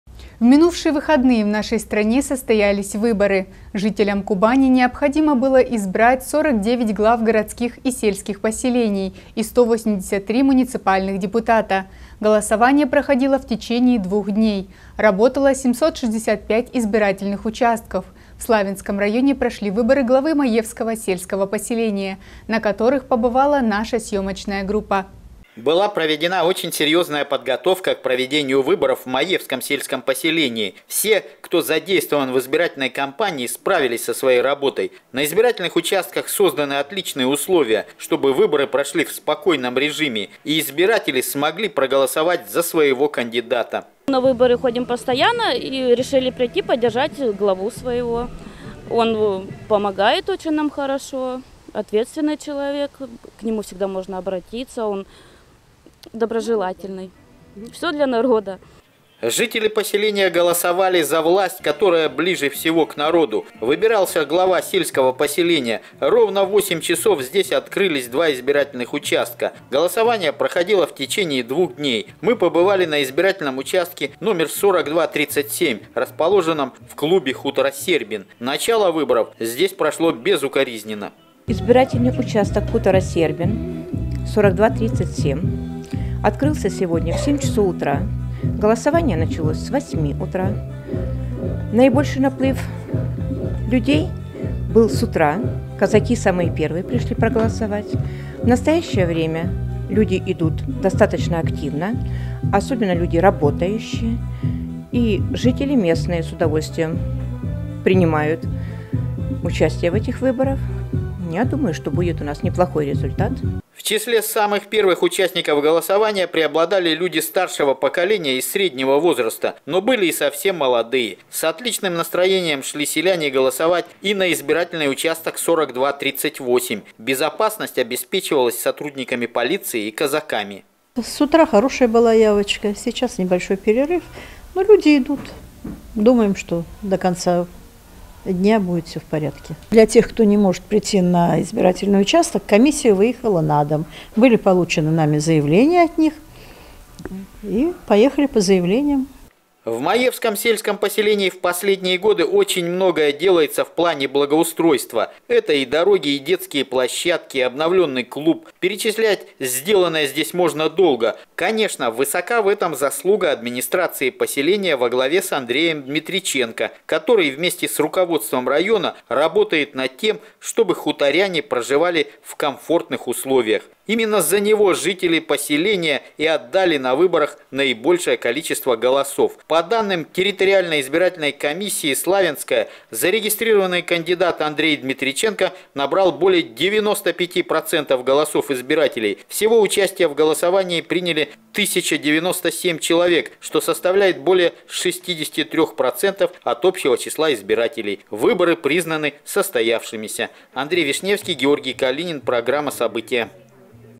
Выборы состоялись
В минувшие выходные в нашей стране состоялись выборы. Жителям Кубани необходимо было избрать 49 глав городских и сельских поселений и 183 муниципальных депутата. Голосование проходило в течение двух дней, работало 765 избирательных участков. В Славянском районе прошли выборы главы Маевского сельского поселения, на которых побывала наша съемочная группа